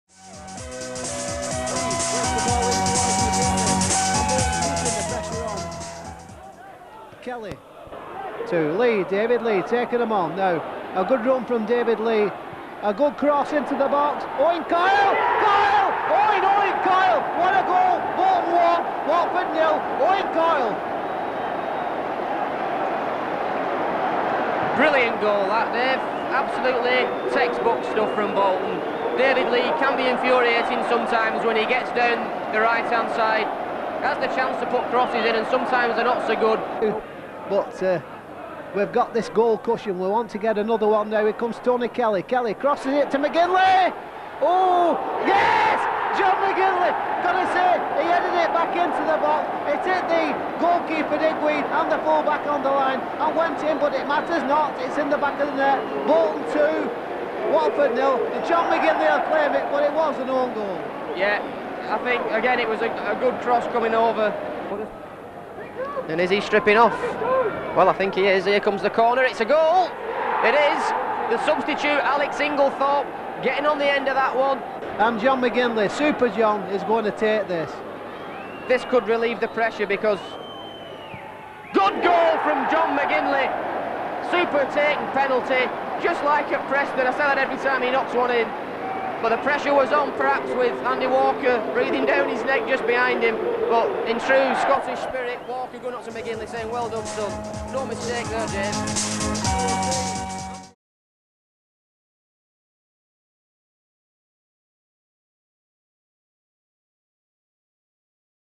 GOLDEN GOALS | Bolton 3-1 Watford (1994)
Watch back classic highlights of Bolton v Watford from 1994, as goals from Owen Coyle, John McGinlay and Mark Watson (og) secured victory for the whites.